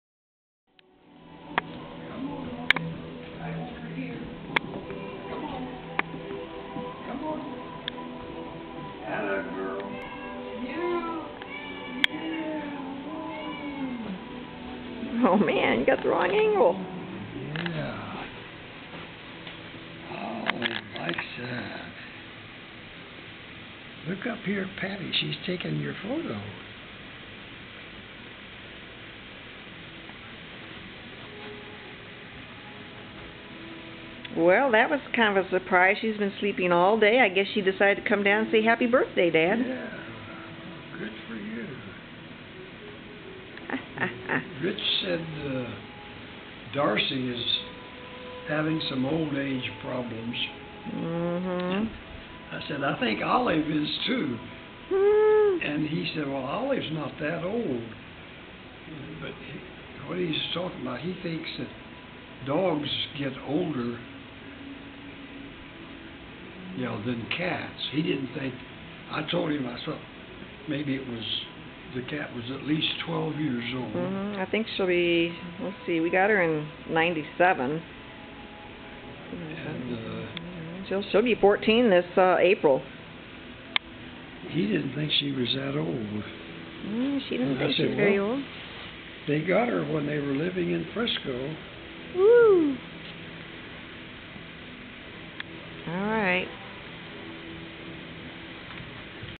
Dad's 90th Birthday - Part 3
Olive decides to say hello to Dad on his 90th Birthday